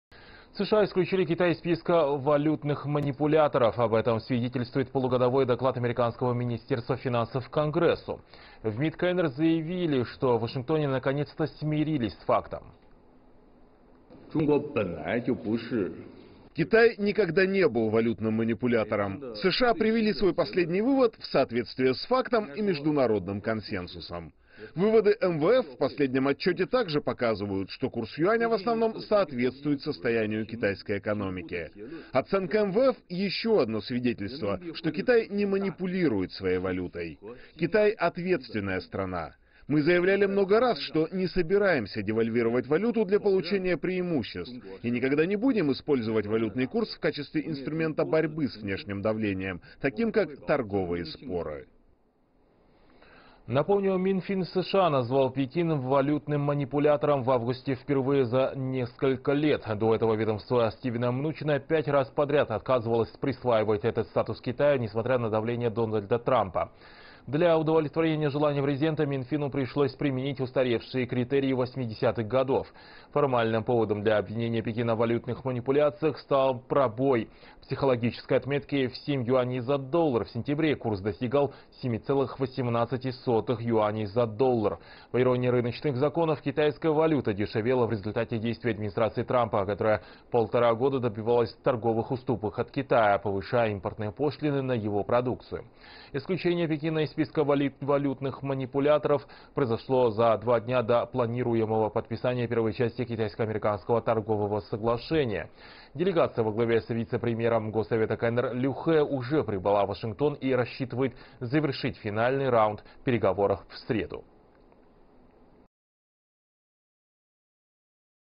Инструмент в торговом споре
США исключили Китай из списка валютных манипуляторов. Об этом свидетельствует полугодовой доклад американского министерства финансов Конгрессу. В МИД КНР заявили, что в Вашингтоне наконец-то смирились с фактом.